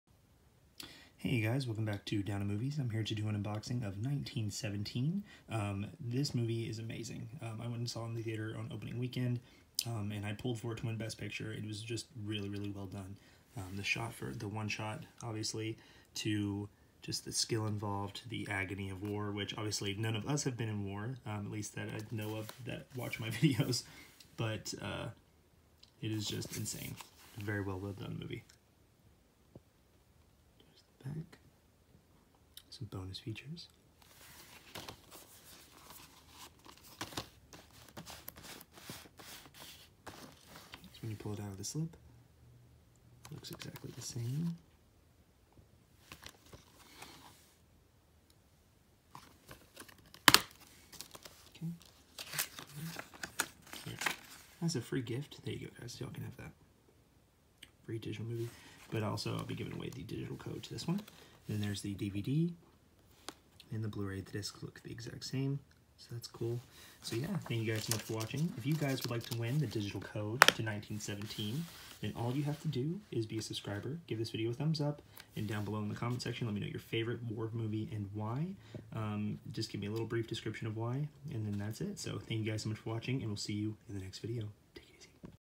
1917 (2019) Blu-ray Unboxing! Plus Digital Code Giveaway!
Hey guys! Here is my unboxing of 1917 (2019) on Blu-ray! I hope you enjoy!:)

2nd Instagram: MovieImposter
3rd Instagram: wallemoviepicks